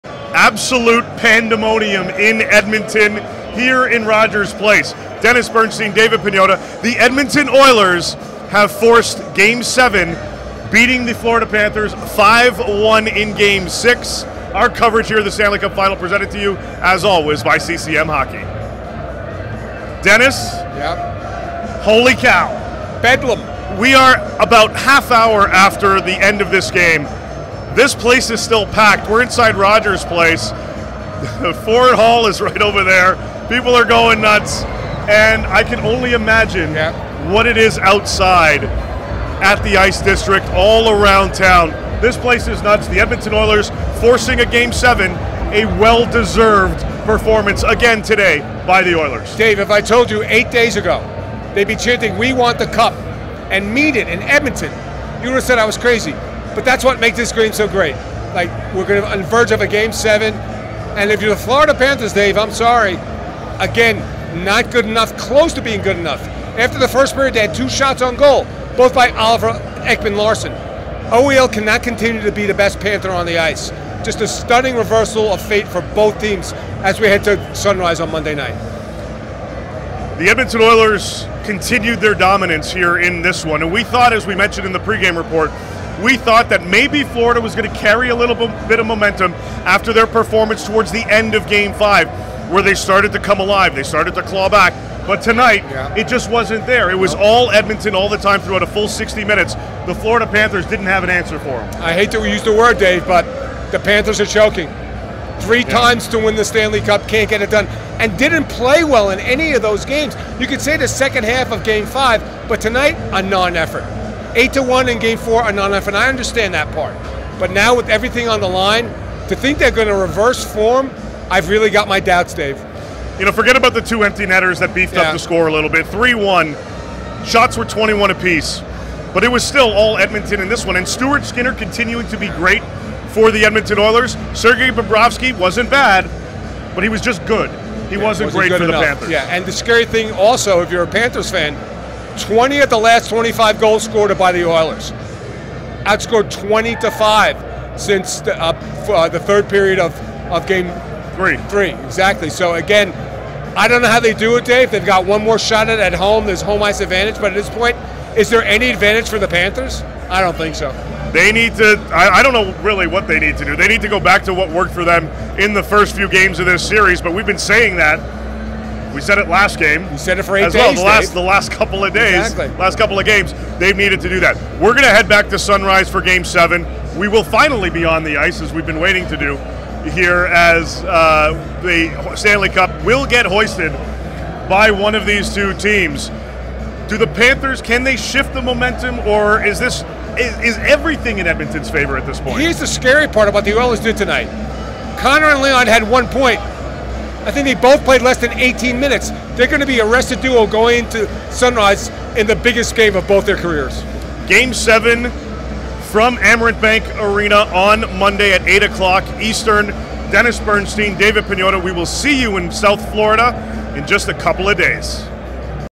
Game 6, Stanley Cup Final: Oilers Force Game 7, Beat Panthers 5-1
Absolute pandemonium in Edmonton as the Edmonton Oilers beat the Florida Panthers 5-1 on Friday night and force a Game 7 on Monday!

David Pagnotta & Dennis Bernstein recap Game 6 from Rogers Place and highlight the Oilers' continued dominance, their depth performance, and discuss what's going on with the Panthers, who have now lost three straight games.